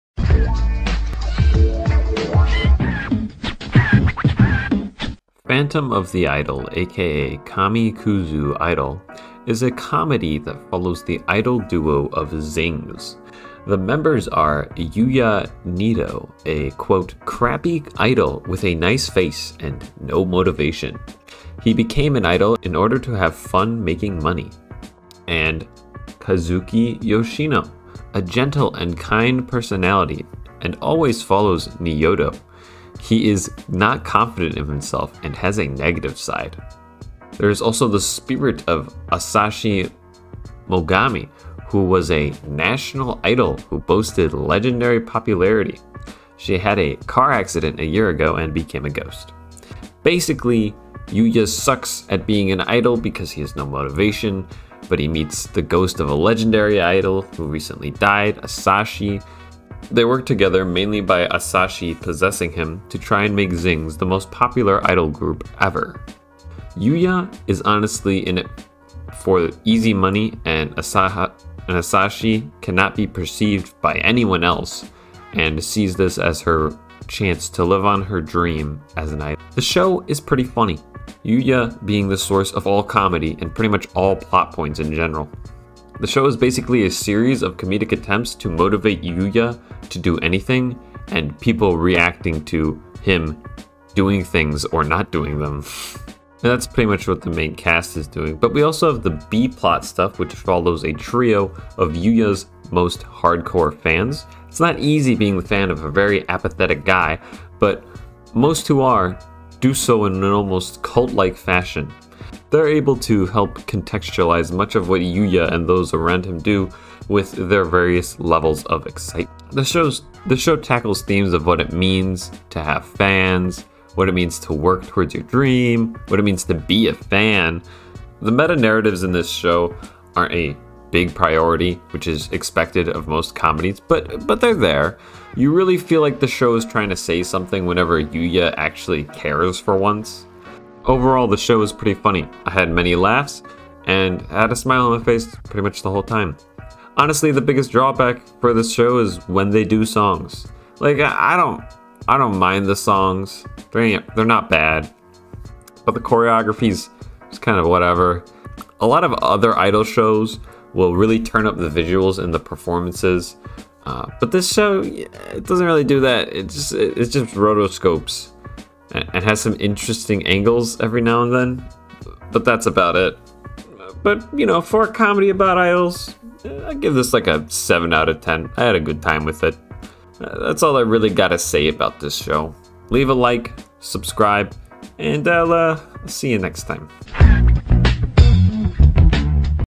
Phantom of the Idol Review (Kami Kuzu☆Idol)
Kazuki Yoshino and Yuuya Niyodo are idols who make up the duo ZINGS. While Kazuki gives his all in every performance, Yuuya views the job as just another means of making money. Yuuya's apathetic attitude toward the fans and inability to take his job seriously causes his manager to give him an ultimatum: unless he starts putting in more effort, he will be fired.

Backstage, Yuuya meets Asahi Mogami, an idol dressed in a colorful costume. Asahi would have had a bright future ahead of her—had she not died a year prior. Even as a ghost, Asahi wants nothing more than to perform for fans again. When she finds out that Yuuya does not hold the same passion for the idol life that she once did, she is overcome by a fit of rage and demands that he let her take his place. In a strange twist, Yuuya wakes up to find that Asahi has possessed him and can do what he is too lazy to do himself.

With Asahi performing for him, Yuuya thinks he has a ticket to an easy idol life. However, Asahi has other plans. Though she is no longer alive, she wishes for Yuuya to find the ambition that she still has. Together, she wants them to become the best idol the world has ever seen. But can an unmotivated man like Yuuya ever care about anything besides money?